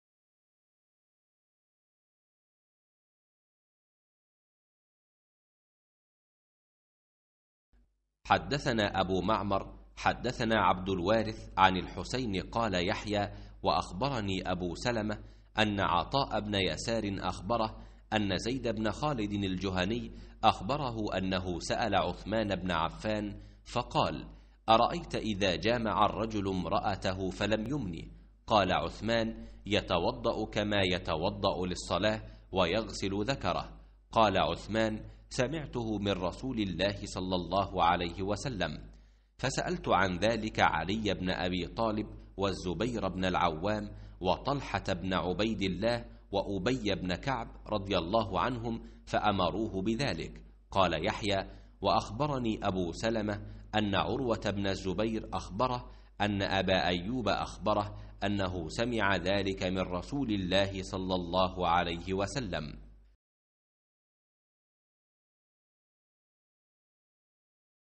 صحيح البخارى - كتاب الغسل - 292 - باب غسل ما يصيب من فرج المرأة
حدثنا أبو معمر، حدثنا عبد الوارث، عن الحسين، قال يحيى وأخبرني أبو سلمة، أن عطاء بن يسار، أخبره أن زيد بن خالد الجهني أخبره أنه، سأل عثمان بن عفان فقال أرأيت إذا جامع الرجل امرأته فلم يمن‏.‏ قال عثمان يتوضأ كما يتوضأ للصلاة، ويغسل ذكره‏.‏ قال عثمان سمعته من رسول الله صلى الله عليه وسلم‏.‏ فسألت عن ذلك علي بن أبي طالب والزبير بن العوام وطلحة بن عبيد الله وأبى بن كعب ـ رضى الله عنهم ـ فأمروه بذلك‏.‏ قال يحيى وأخبرني أبو سلمة أن عروة بن الزبير أخبره أن أبا أيوب أخبره أنه سمع ذلك من رسول الله صلى الله عليه وسلم‏.‏